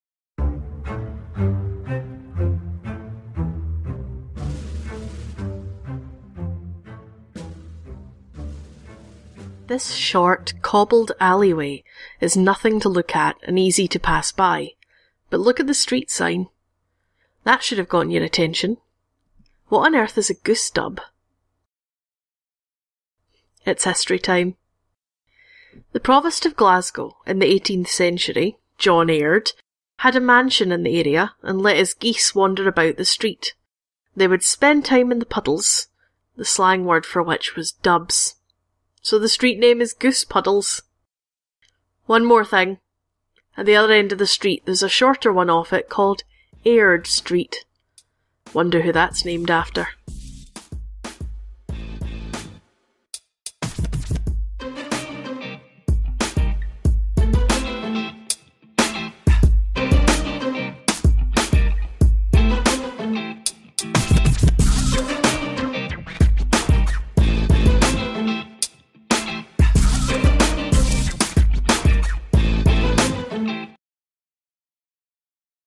Streets of Glasgow 3: Goosedubbs
Even smaller than our "Little Wanders" series, the Streets of Glasgow takes a tiny look at individual streets - this time, Goosedubbs.